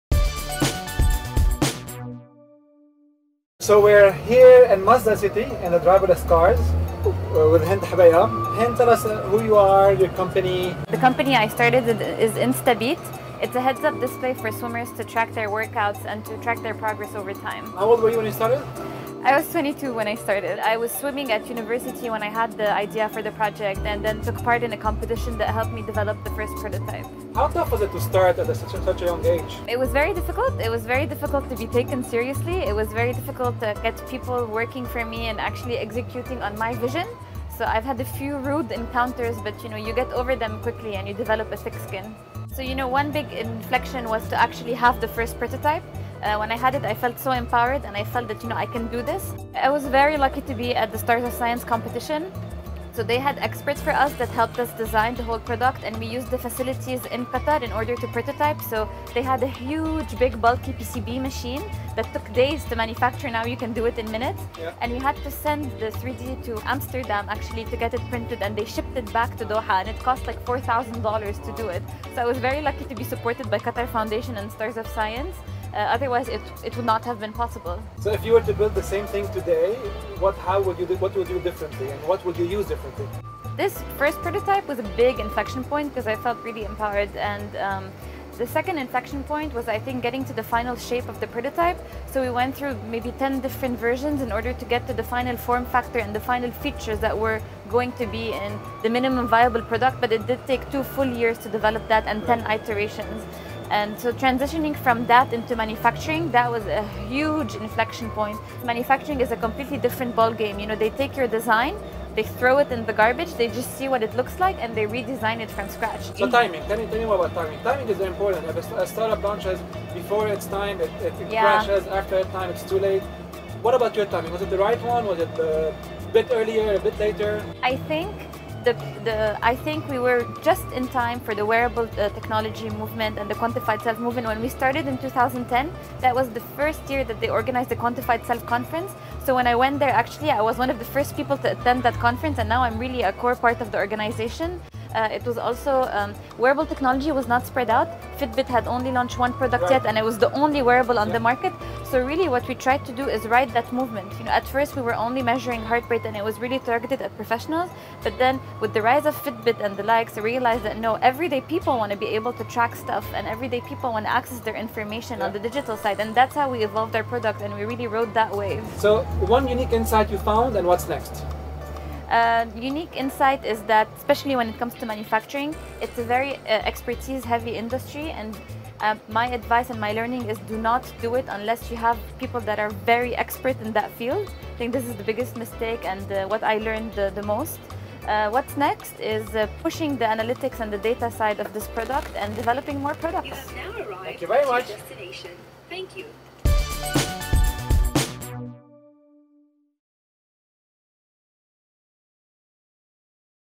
Hind Hobeika discusses manufacturing challenges [Arabic subtitles]
Wamda's cofounder and CEO, Habib Haddad interviewed in the video above, Hind Hobeika, founder of Instabeat, from a driverless car in Masdar City, Abu Dhabi.

Instabeat is a heads up display for swimmers to track their workouts and their progress over time. Hobeika discussed challenges related to team building, manufacturing and funding.